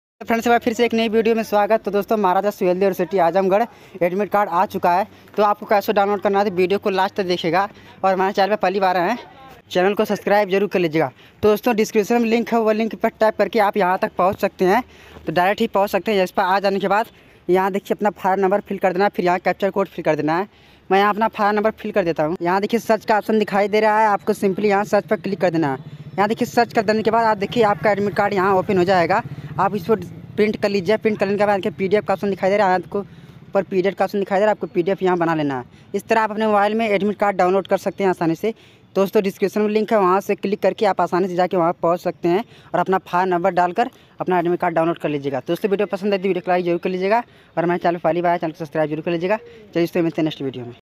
1st & 3rd Semester Ka Admit Card kaise download karen Maharaja suheldev University Azamgarh
1st & 3rd Semester Ka Admit Card kaise download karen Maharaja suheldev University Azamgarh